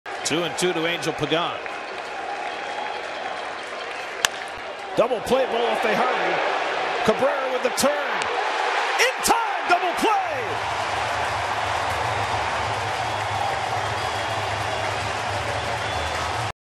SF@NYM: Familia induces a key 4-6-3 double play
Jeurys Familia forces Angel Pagan to ground into a 4-6-3 double play to get the first two outs in the top of the 9th inning